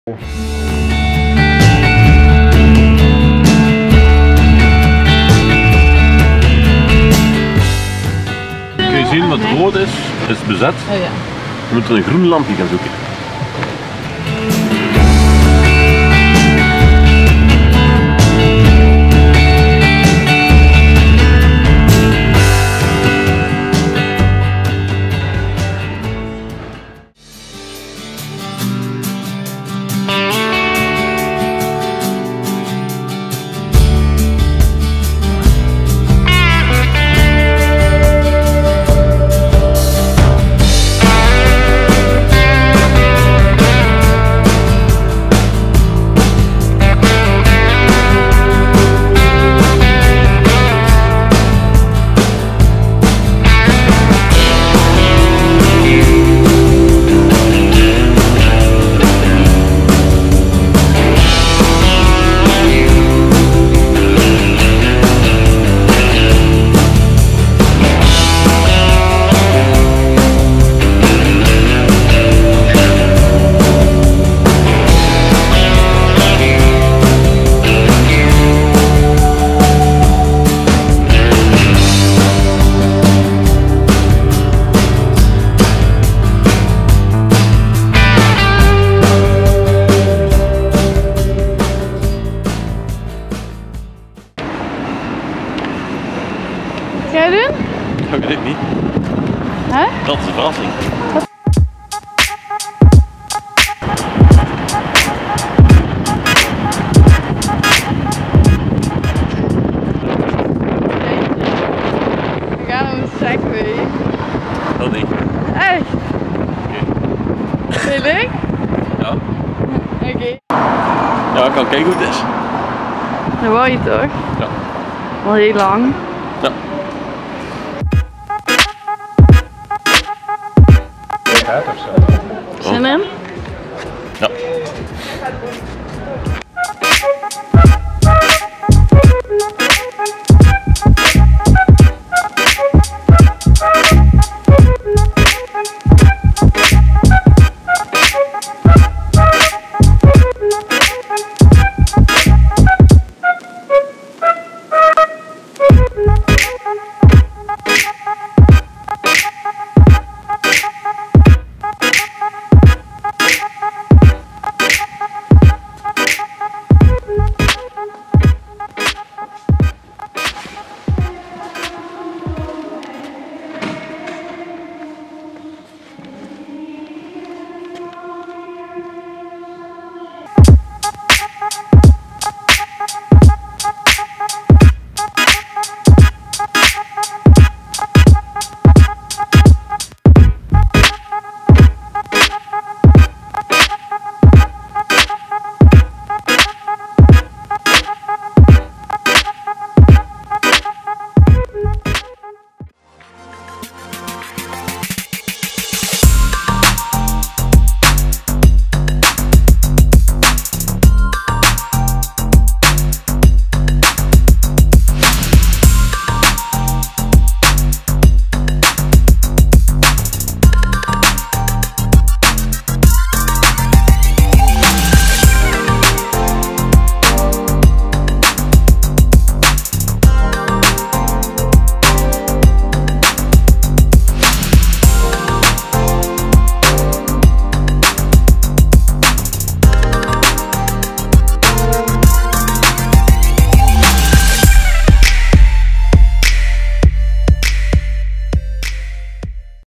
Citytrip Bruges / "Brugge" | Segway Tour | Bruges by night
Join us on our day in Bruges (spelled "Brugge" in Dutch) – one of Belgium's most popular medieval travel destinations. It's one of the most beautiful and romantic city's of Europe.

Walking trough Bruges. Segway Tour and Bruges by night.

Please let us know your thoughts about this video.